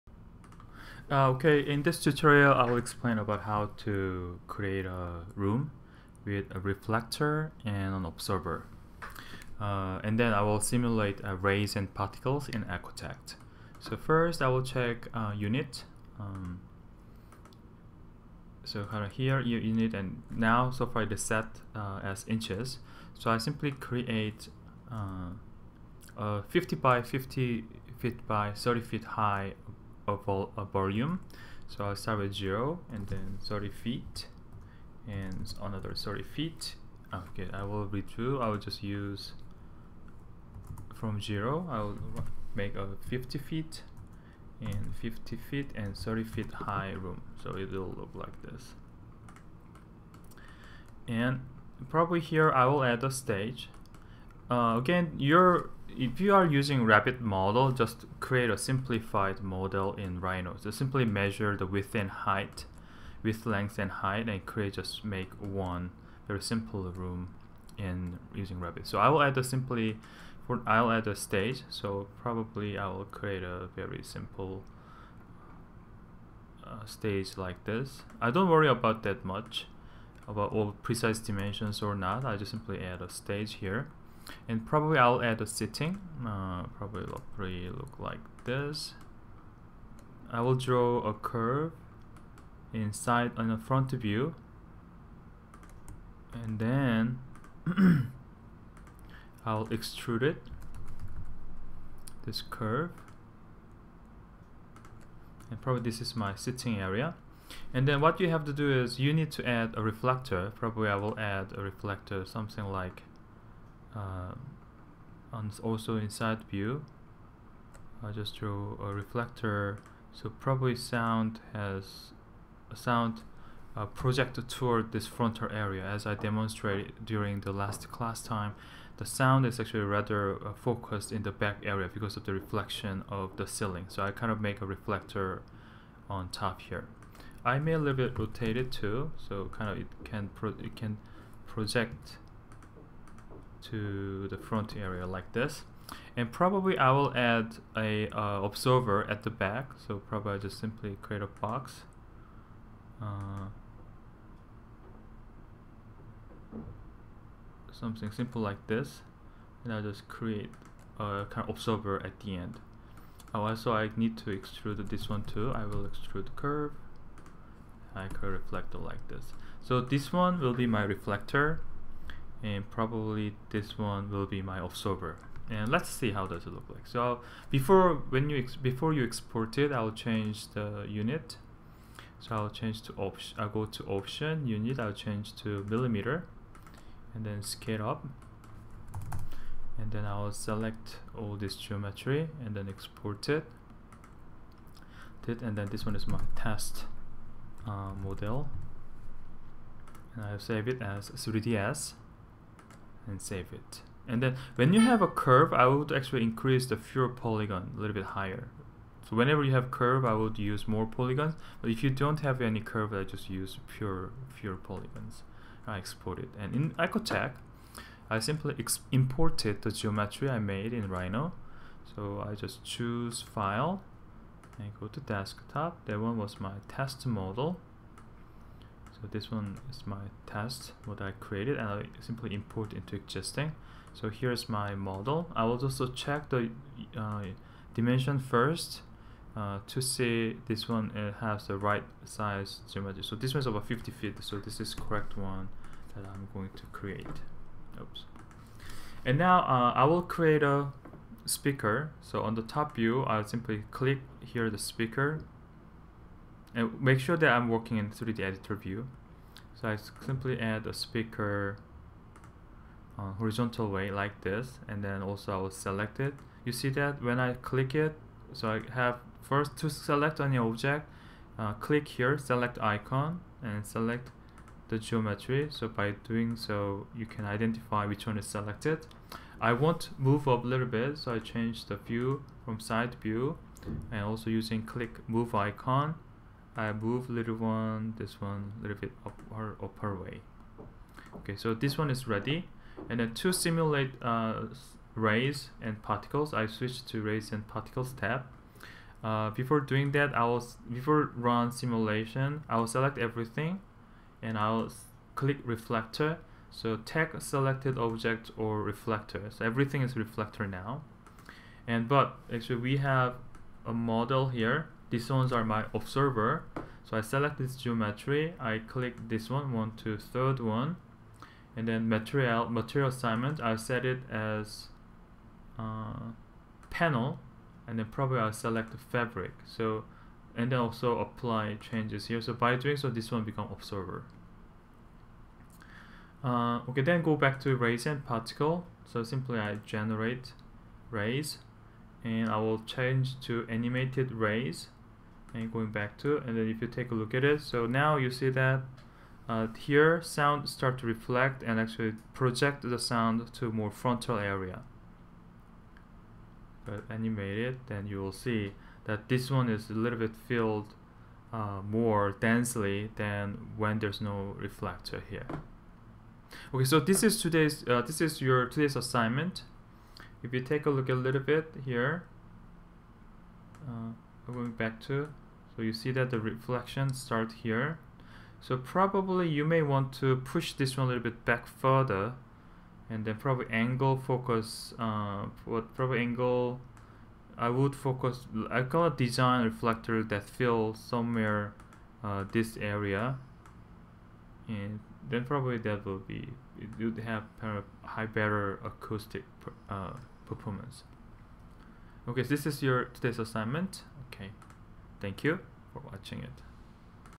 Simulate Rays with a reflector and an absorber
ARC 363 Lighting and Acoustic Spring 2015
Assistant Professor Ju Hong Park
Week 13 Ecotect Rays and Particles Simulation.